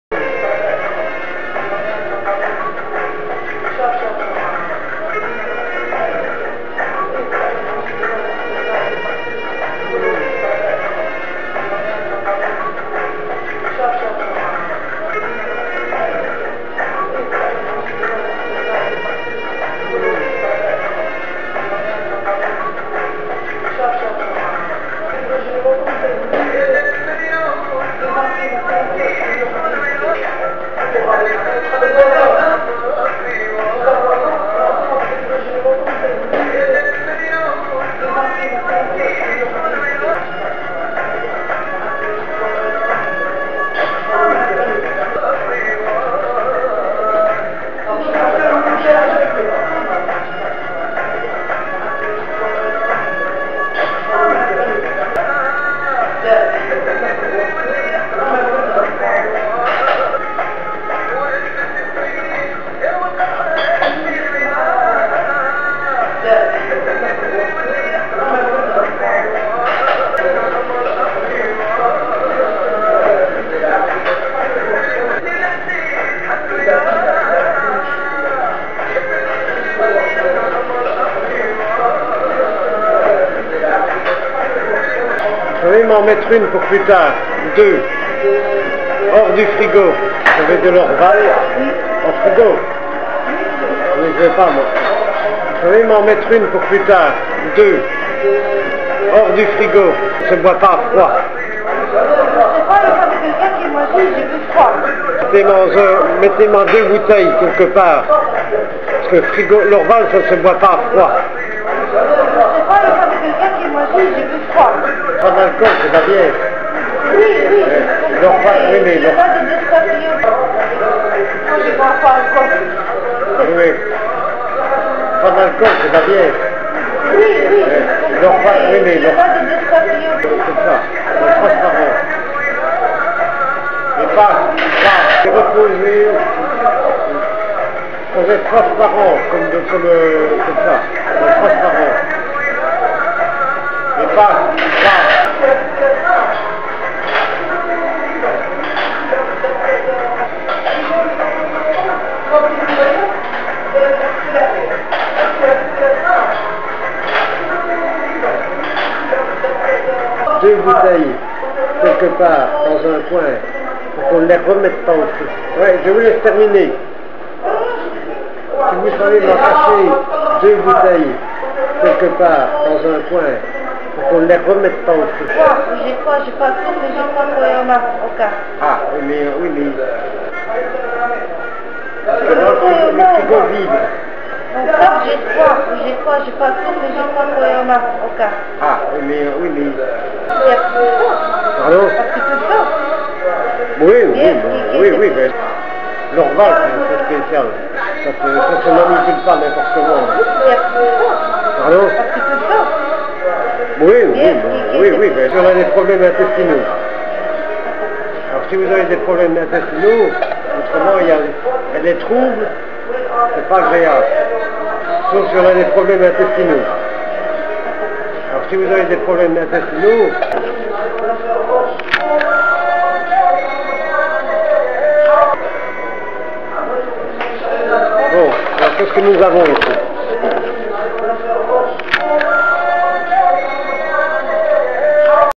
MON_FRERE_JUMEAU
Vous connaissez mon frère jumeau.
Comme les Dupont et d , il répète tout ce que je dis0